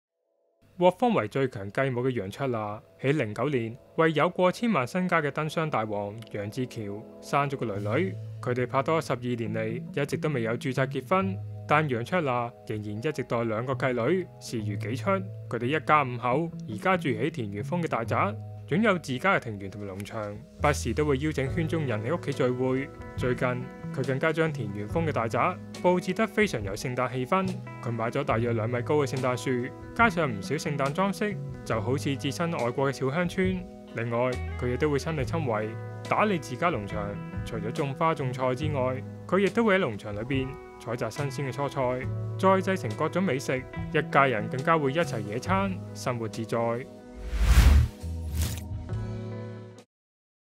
楊卓娜佈置田園風大宅設2米高聖誕樹 氣氛濃彷如置身外國小鄉村
獲封為「最強繼母」的楊卓娜在2009年為有過千萬身家的「燈箱大王」楊志翹誕下女兒楊舒然（豹豹），拍拖12年來一直未有註冊結婚，但楊卓娜依然一直待兩名繼女視如己出，組成五口之家，一家五口現居於一間田園風大宅，擁有自家庭院及農場，不時都會邀請圈中人在家聚會。最近她更將田園風大宅佈置得非常有聖誕氣氛，楊卓娜買下約2米高的聖誕樹，加上不少聖誕裝飾，彷如置身外國的小鄉村。